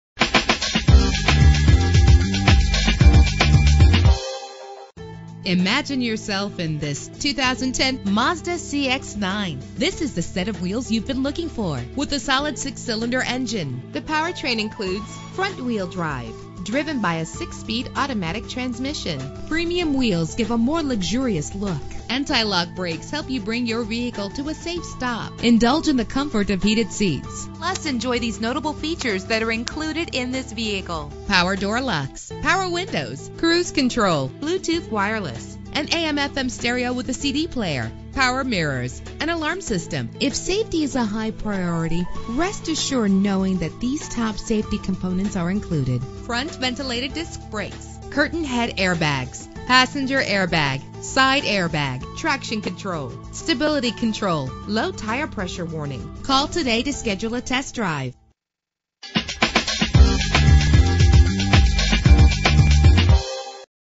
2010 Mazda CX-9 - Stafford TX
Year: 2010
Make: Mazda
Model: CX-9
Trim: Sport
Engine: 3.7 liter 6 cylinder 24 valve
Transmission: 6-Speed Automatic
Color: Dolphin Gray Mica
Mileage: 22717
Address:
12053 Southwest Freeway